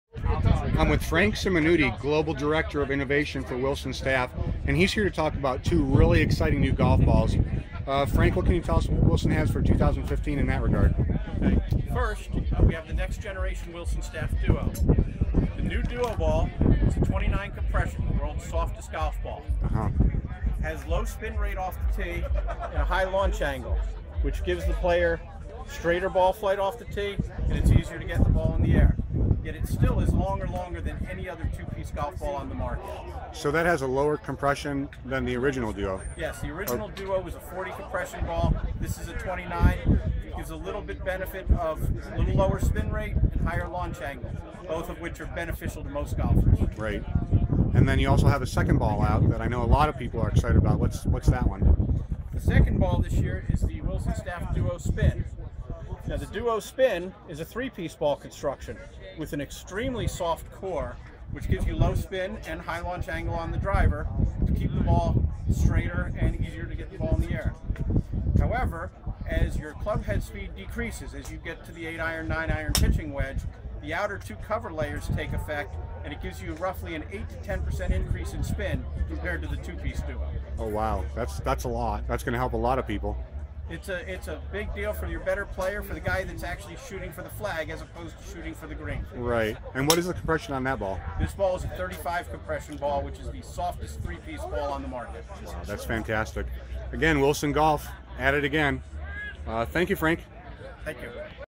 Wilson Staff DUO Spin Golf Balls 2015 PGA Merchandise Show Frank Simonutti
I caught up with Mr. Frank Simonutti, Director of Innovation at Wilson Staff to talk about the new Wilson DUO and DUO Spin golf balls.

Frank designs all Wilson golf balls, and the new DUO balls are one of the big stories of the 2015 PGA Merchandise Show.

You can follow Pete on twitter @PGAPappas for more golf news, reviews, interviews, and giveaways.